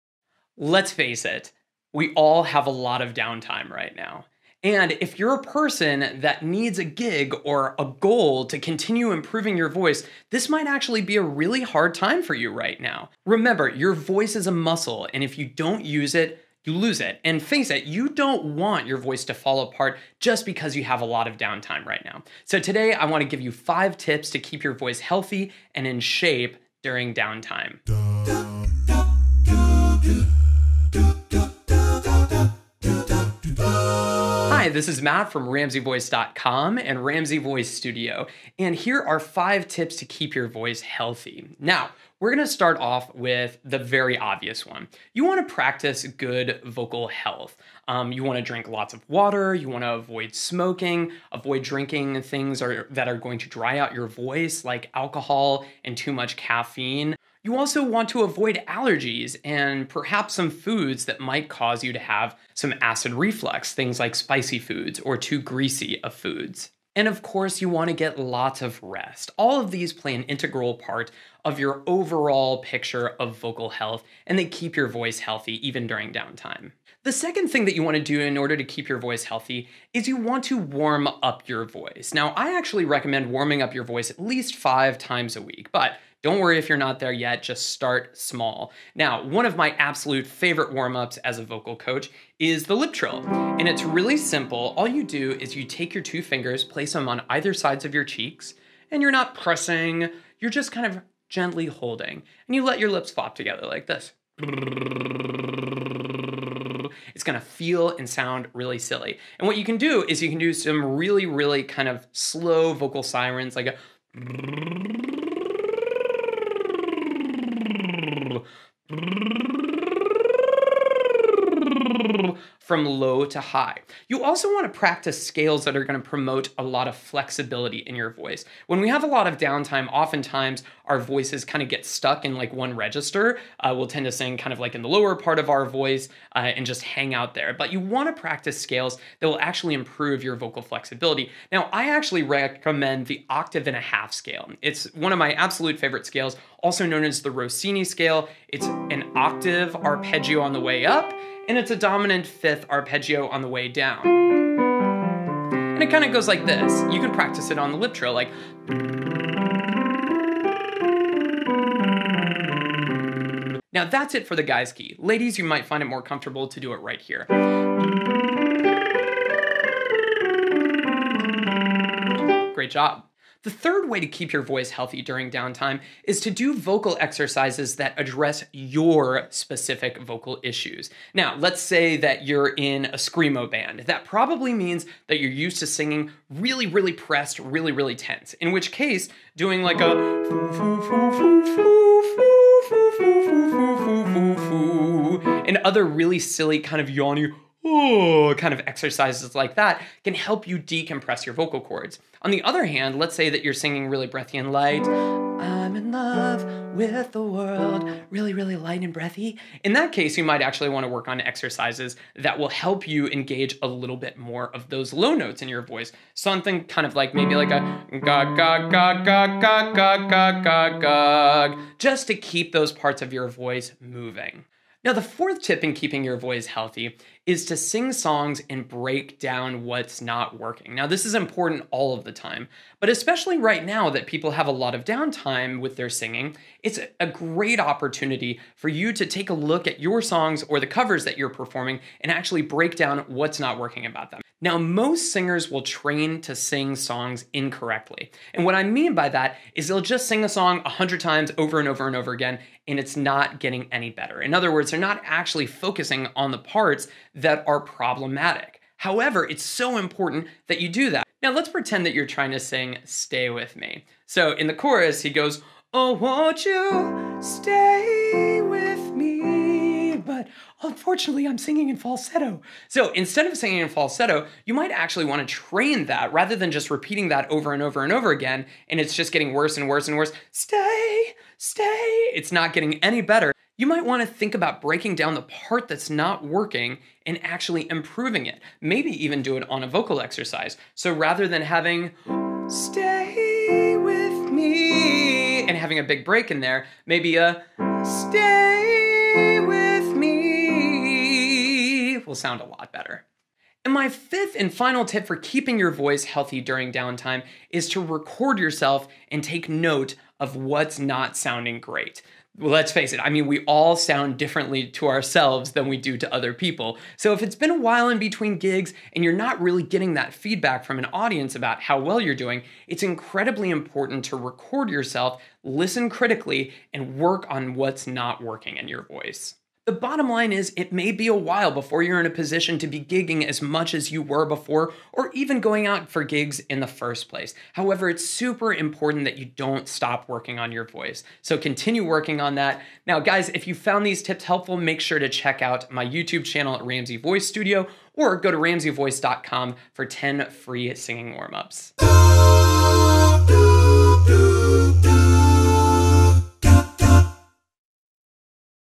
Singing Tips: 5 Ways to Keep Your Voice Healthy During Downtime
Vocal coach Matt Ramsey reminds us that your voice is a muscle that needs regular exercise to stay “fit.” In this video, Matt provides a number of helpful tips on keeping your voice in shape when you don’t have a lot of opportunities to perform. This is the first in a series of Singing Tips videos from Matt for CD Baby, so be sure to check back later for future installments. In the meantime head over to his Youtube channel for more great stuff

Also, make sure to check out this FREE Vocal Technique for Beginners PDF to take your art to next level!